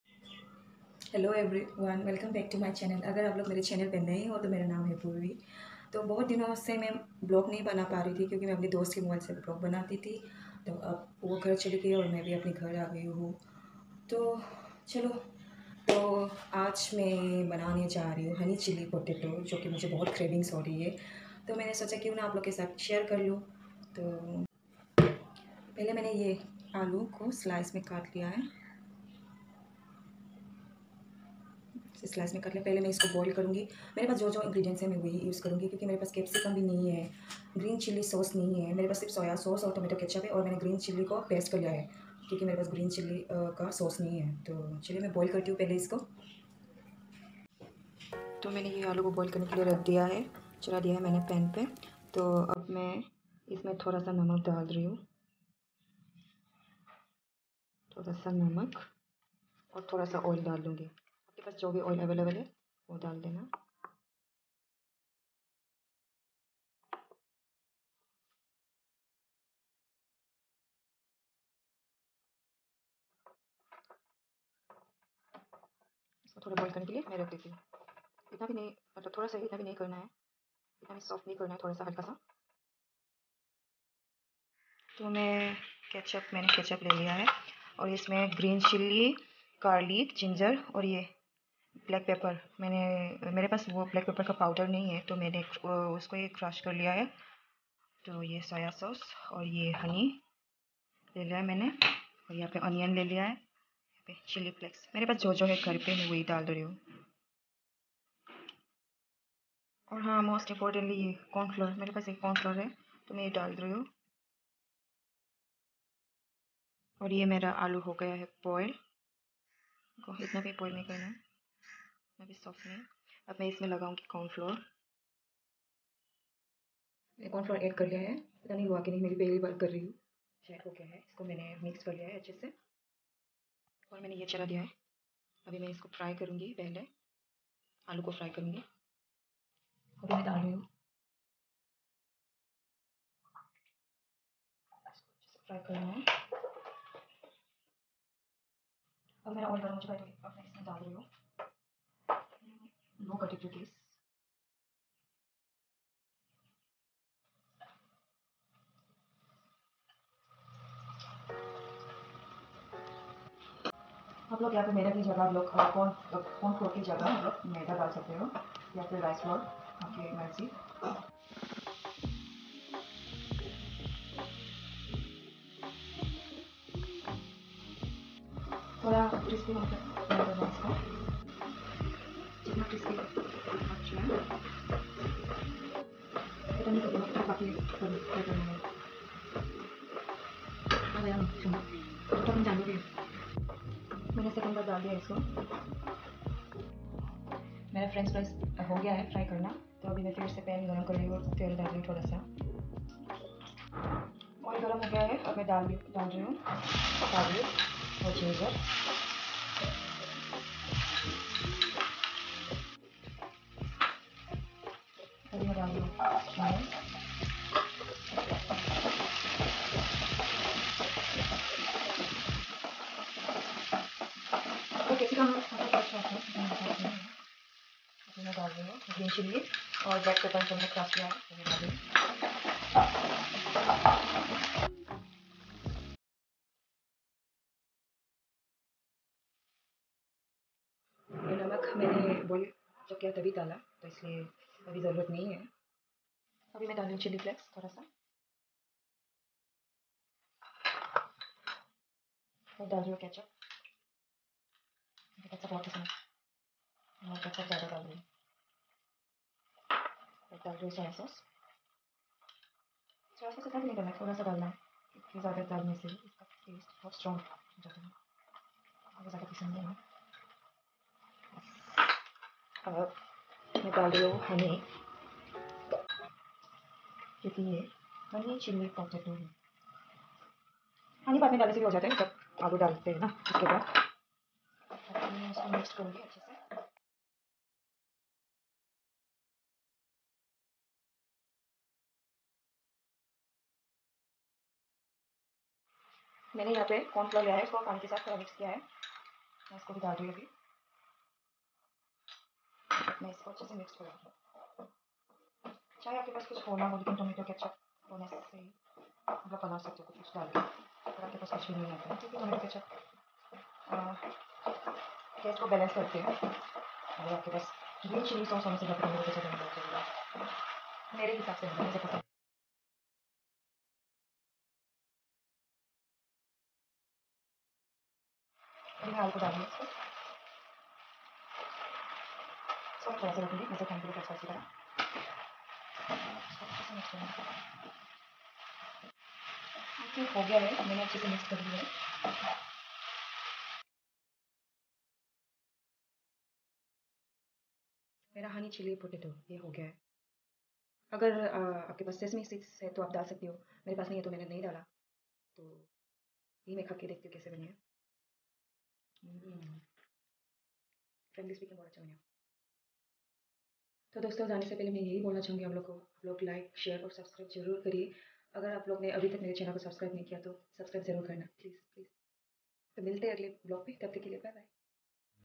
COOKING HONEY🍯 CHILLI🌶️ POTATO🥔 AT HOME/ LOCKDOWN PERIOD/ NORTHEAST BODO GIRL/ EPISODE-5
I TRIED COOKING HONEY CHILLI POTATO AT HOME FOR THE VERY FIRST TIME  WITH VERY LESS INGREDIENTS.